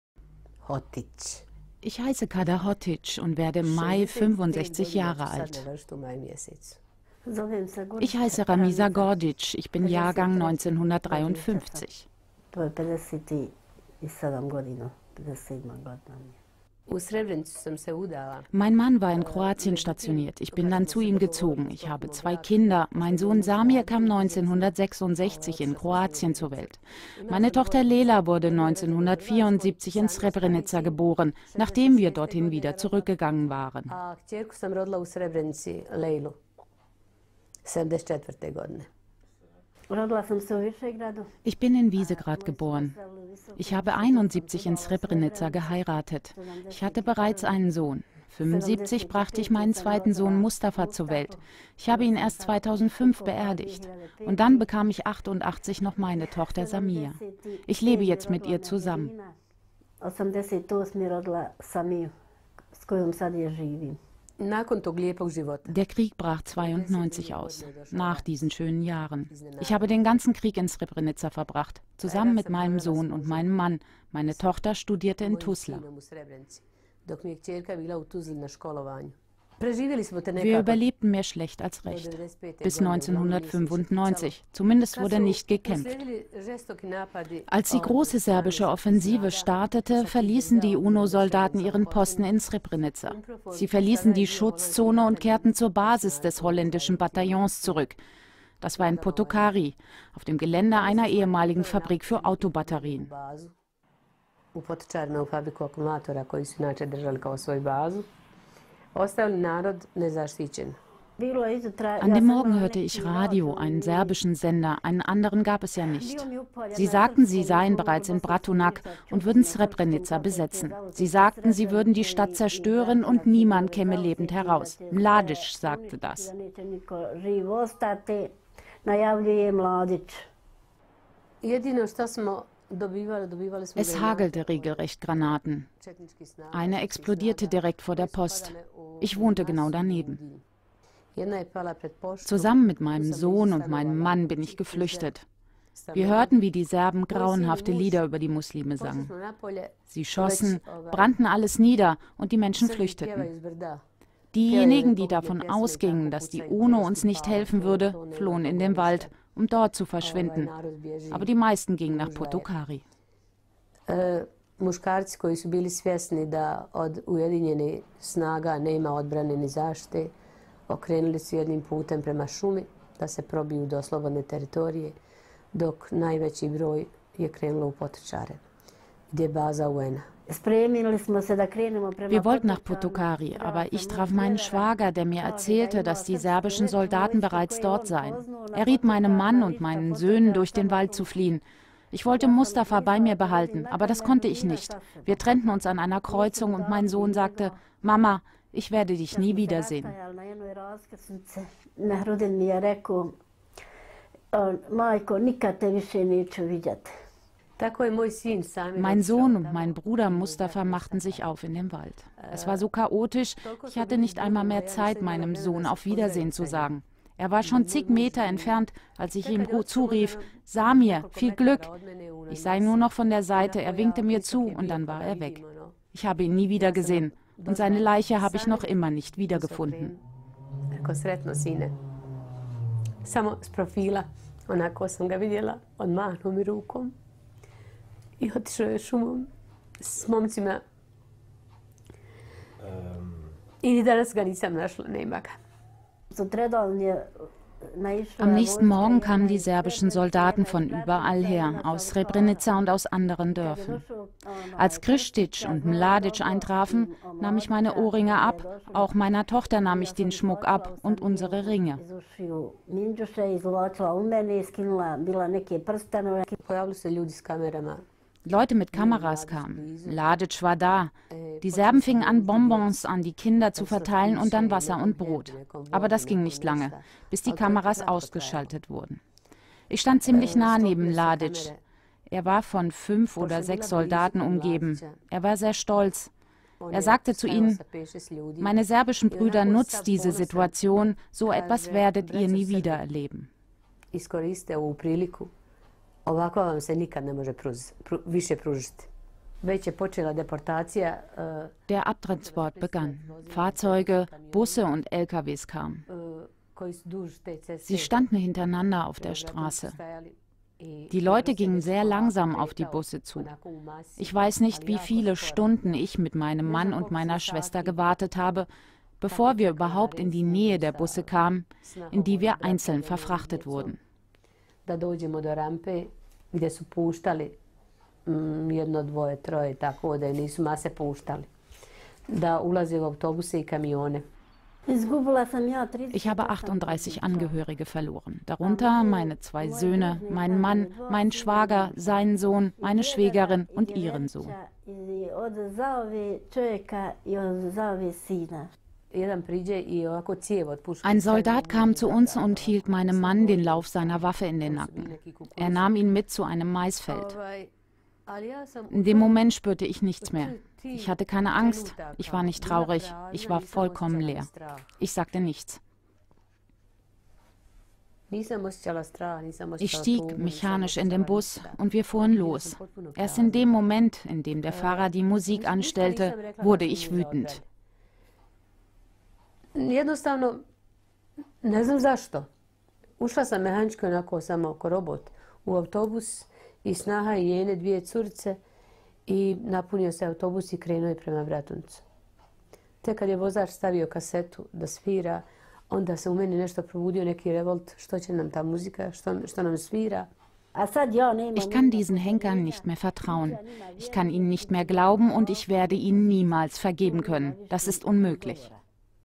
Srebrenica: "Mama ich werde dich nie wiedersehen" (Bosnien)
Kada Hotic und Ramiza Gordic waren vor 15 Jahren in Srebrenica. Ihre Söhne, Ehemänner, Brüder wurden damals von den Serben getötet. Seit Jahren engagieren sich die beiden bei den "Müttern von Srebrenica". Was damals geschah, haben sie Yannick Cador und Emmanuel Royer erzählt.

Kada Hotic:
„Der Krieg brach 92 aus, nach diesen schönen Jahren. Ich habe den ganzen Krieg in Srebrenica verbracht, zusammen mit meinem Sohn und meinem Mann. Meine Tochter studierte in Tuzla.“
„Wir überlebten mehr schlecht als recht bis 1995. Zumindest wurde nicht gekämpft. „

Kada Hotic:
„Als die große serbische Offensive startete, verließen die UNO-Soldaten ihren Posten in Srebrenica. Sie verließen die Schutzzone und kehrten zur Basis des holländischen Bataillons zurück. Das war in Potocari, auf dem Gelände einer ehemaligen Fabrik für Autobatterien.“

Ramiza Gordic:
„An dem Morgen hörte ich Radio, einen serbischen Sender, einen anderen gab es ja nicht. Sie sagten, sie seien bereits in Bratunac und würden Srebrenica besetzen. Sie sagten, sie würden die Stadt zerstören und niemand käme lebend heraus. Mladic sagte das.“

Kada Hotic:
„Es hagelte regelrecht Granaten. Eine explodierte direkt vor der Post, ich wohnte genau daneben. Zusammen mit meinem Sohn und meinem Mann bin ich geflüchtet. Wir hörten, wie die Serben grauenhafte Lieder über die Muslime sangen. Sie schossen, brannten alles nieder, und die Menschen flüchteten. Diejenigen, die davon ausgingen, dass die UNO uns nicht helfen würde, flohen in den Wald, um dort zu verschwinden, aber die meisten gingen nach Potocari.“

Ramiza Gordic:
„Wir wollten nach Potocari, aber ich traf meinen Schwager, der mir erzählte, dass die serbischen Soldaten bereits dort seien. Er riet meinem Mann und meinen Söhnen, durch den Wald zu fliehen. Ich wollte Mustafa bei mir behalten, aber das konnte ich nicht. Wir trennten uns an einer Kreuzung und mein Sohn sagte: Mama, ich werde dich nie wiedersehen.“